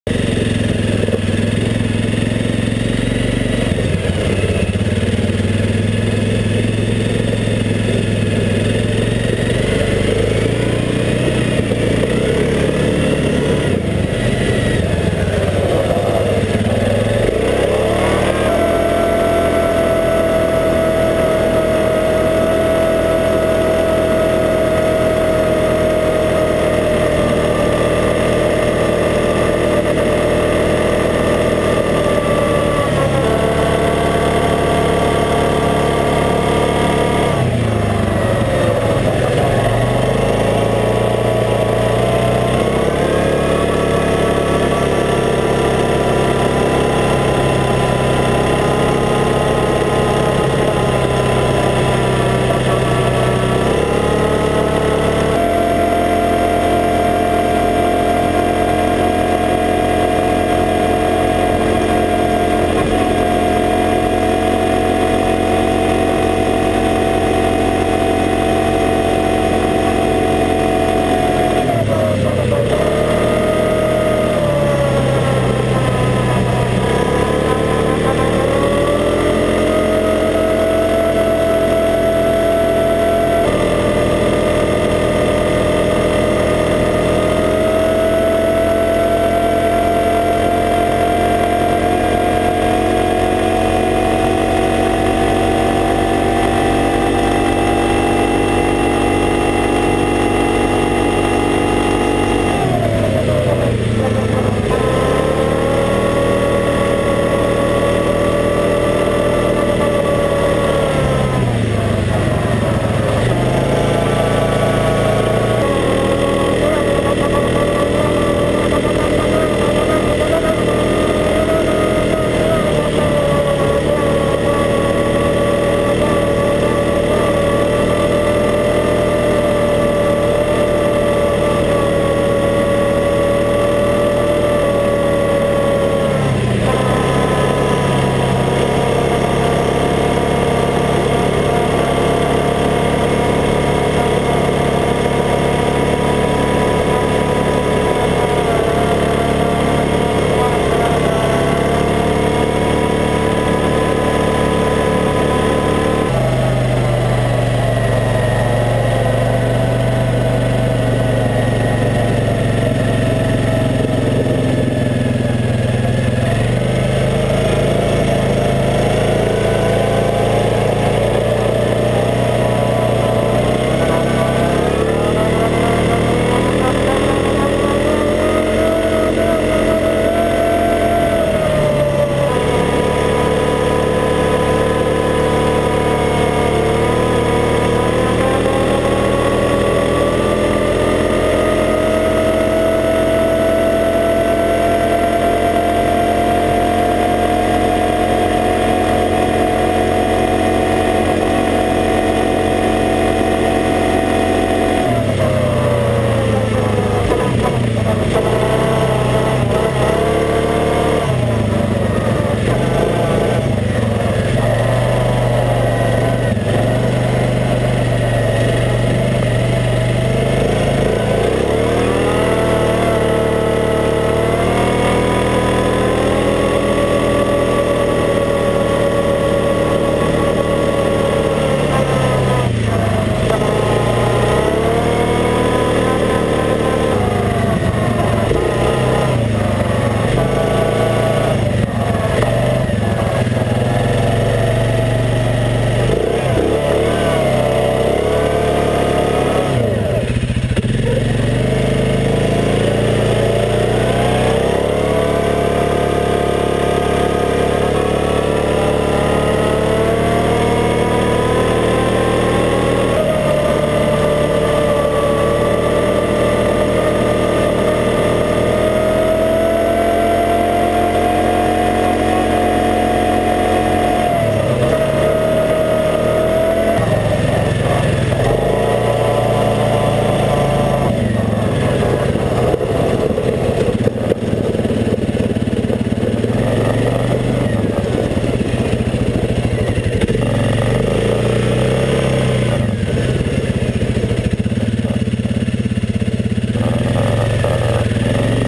1 25 2014 Quading on Swains Lake Go Pro Ground View
mounted the go pro where the winch would go.